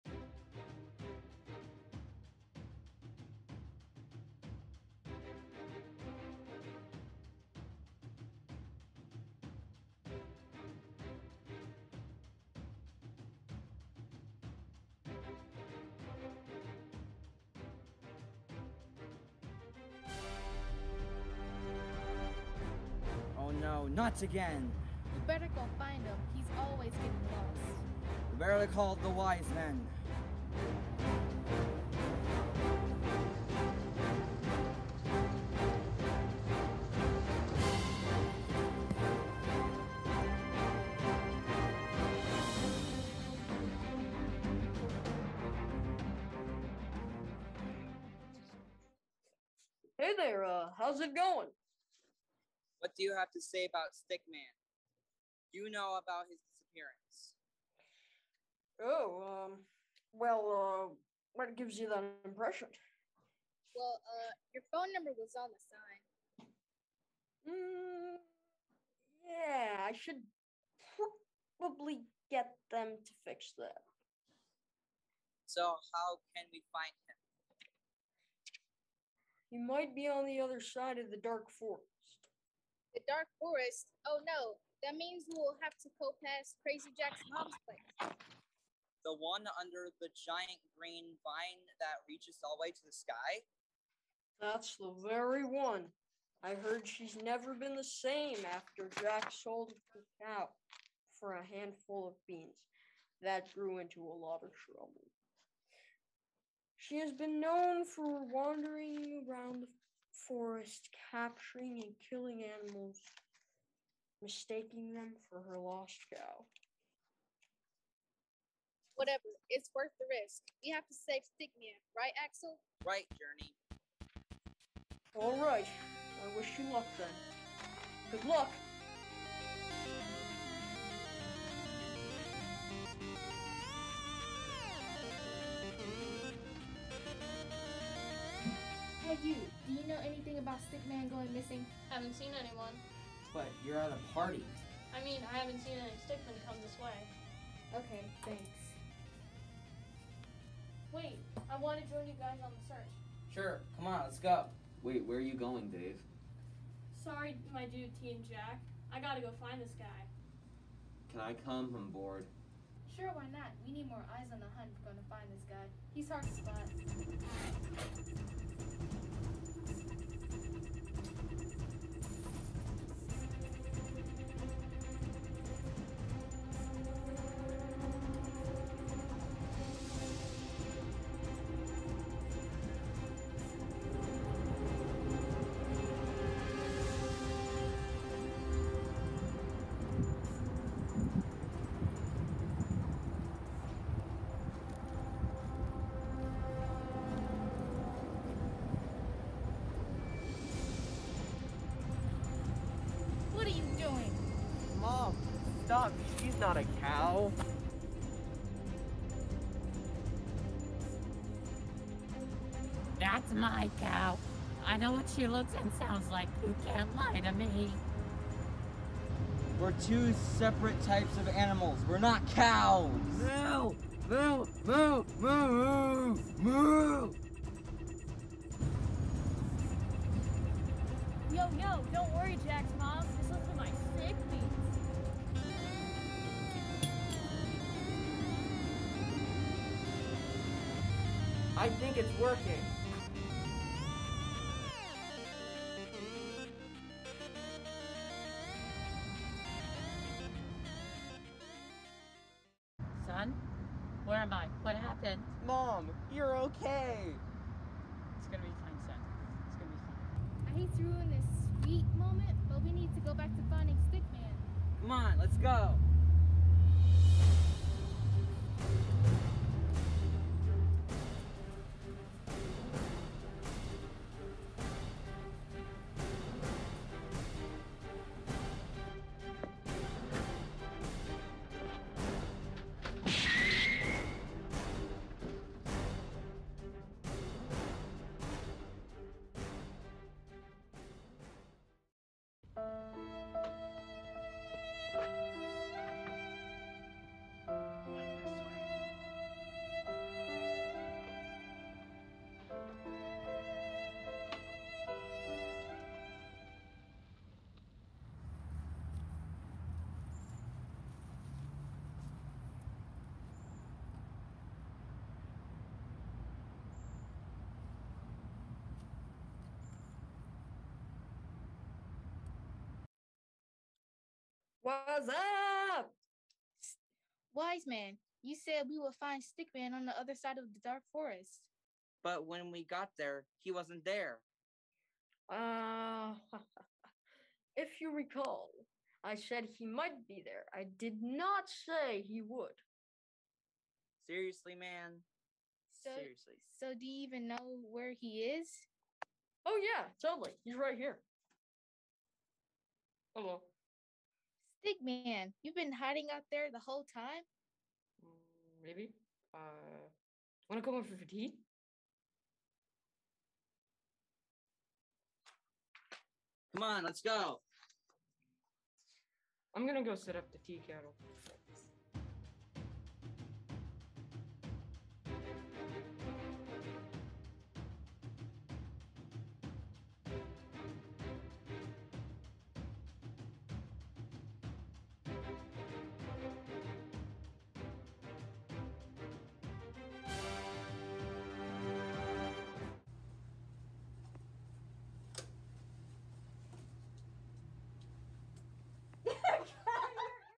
Finding Stickman
This story tells of the trials and tribulations of Axel and Journey, two animal assassin/ninja heroes, as they attempt to locate the once-again missing and often elusive Stickman. Written by Paris, Jax, Landon, and Mrs. B. Filmed by Ayden. Starring Paris, Landon, Jax, Braydan, Justin, and Adrian. Edited by Mrs. B and Paris.